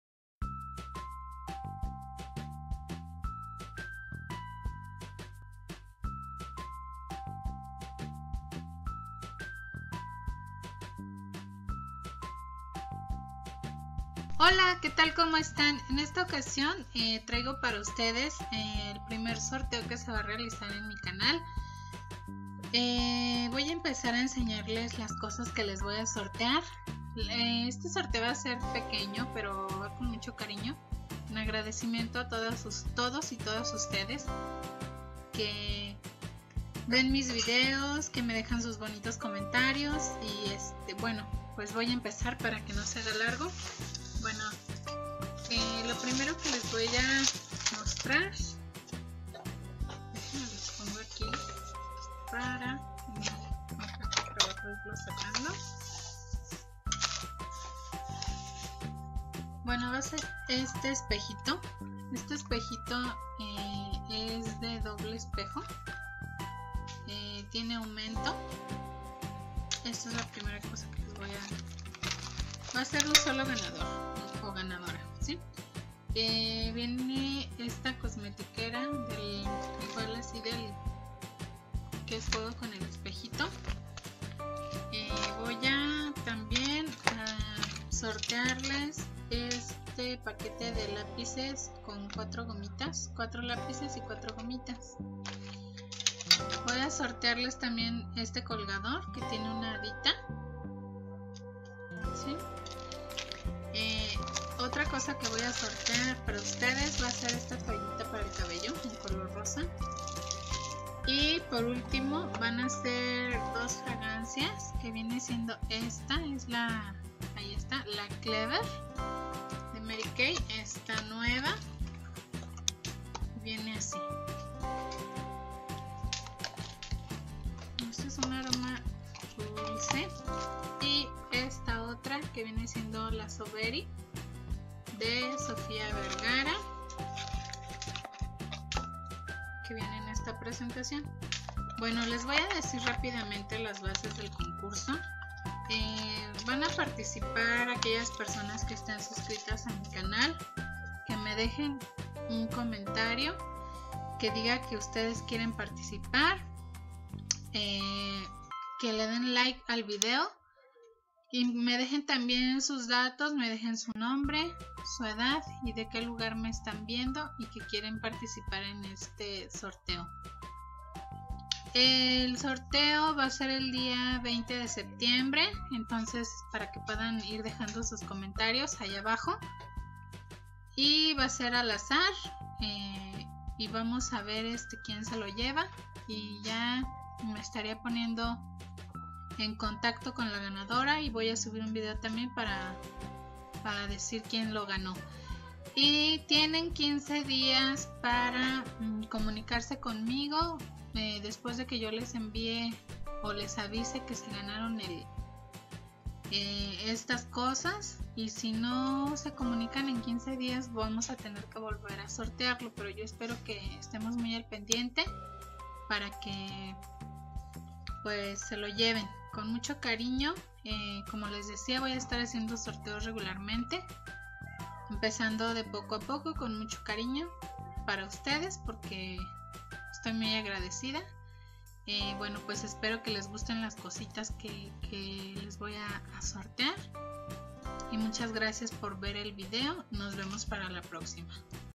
PRIMER SORTEO!!! 20 DE SEPTIEMBRE 2016
PRIMER SORTEO QUE SERA REALIZADO CON MUCHO CARIÑO
BASES:
ESTAR SUSCRITO A MI CANAL Y DEJAR UN COMENTARIO CON TUS DATOS
DAR LIKE AL VIDEO
NOMBRE, EDAD Y LUGAR DONDE VIVES
SI NO ERES MAYOR DE EDAD CONTAR CON LA AUTORIZACION DE TUS PADRES
EL SORTEO SERA EL DIA 20 DE SEPTIEMBRE
TIENES 15 DIAS PARA CONTESTAR MI MENSAJE PARA PODER ENVIARTE EL PREMIO DE LO CONTRARIO SERA SORTEADO NUEVAMENTE
MUCHA SUERTE!!!